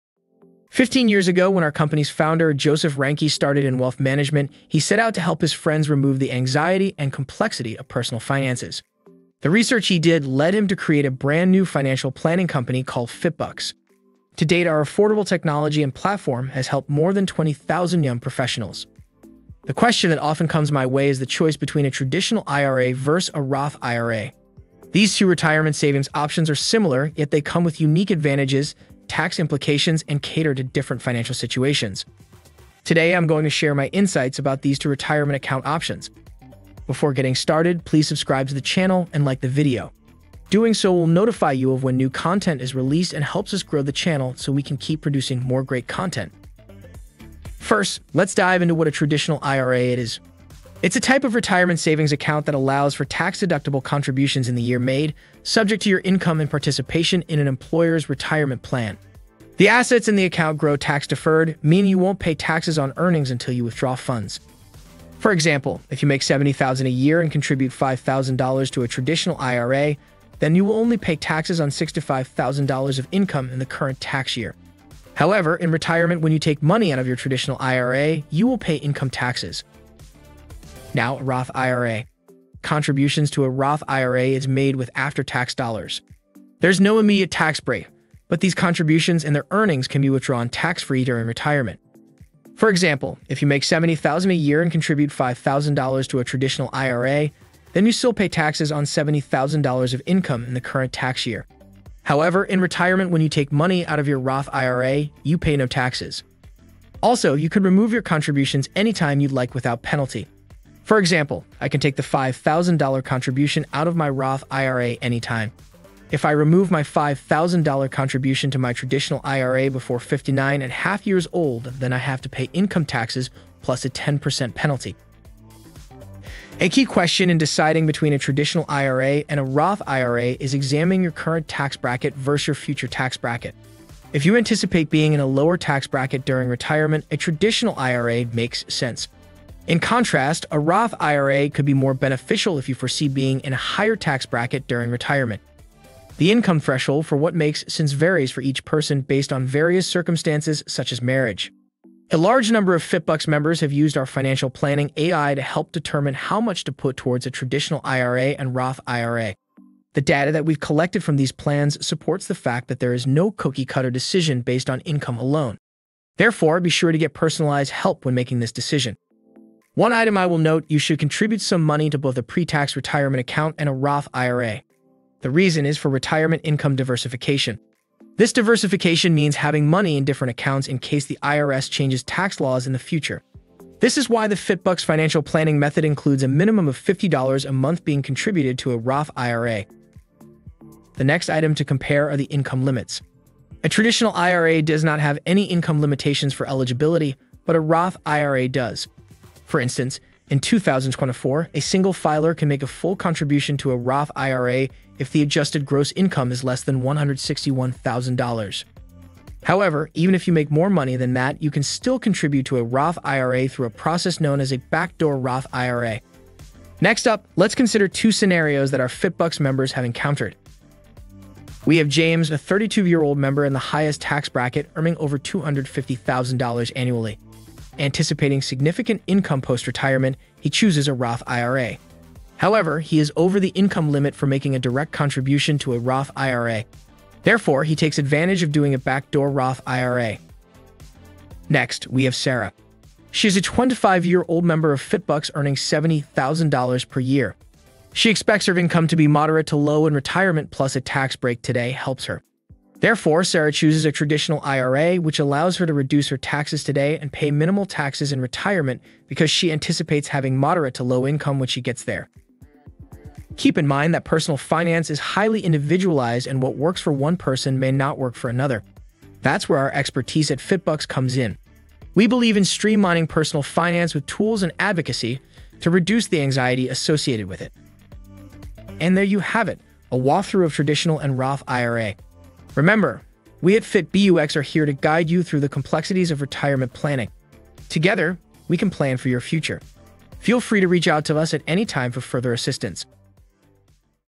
Traditional vs Roth IRA: Quick Reference Guide For Retirement Planning
Join us as we dive deep into the world of retirement savings with our quick reference guide on a Traditional IRA vs  Roth IRAs.

In this video, we'll unravel the complexities of each option, discussing eligibility, tax implications, withdrawal rules, and much more.

Whether you're just starting your retirement planning or looking to optimize your existing strategy, this video is packed with expert insights and practical tips to help you make the best decision for your financial future.

Don't miss out on our interactive examples and real-life scenarios that bring these concepts to life!

Chapters:
00:00 Intro
00:49 Traditional IRA
01:20 Roth IRA
02:05 Choosing between a Traditional IRA and Roth IRA
03:13 IRA Income Limits
03:44 Examples